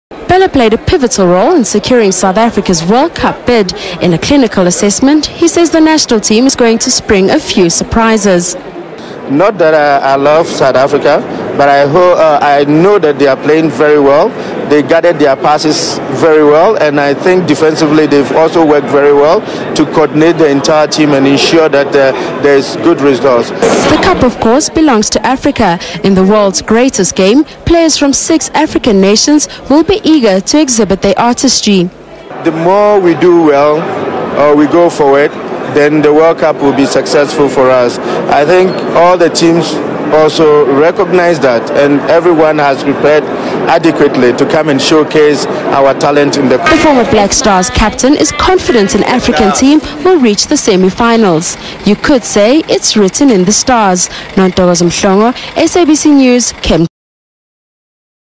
Abedi Pele: Bafana Bafana can cause major upset at FWC 2010
One of Africa's greatest football exports believes Bafana Bafana can cause a major upset during the World Cup...Abedi Pele whose two sons will play for Ghana, has keenly monitored the team's progress in recent months...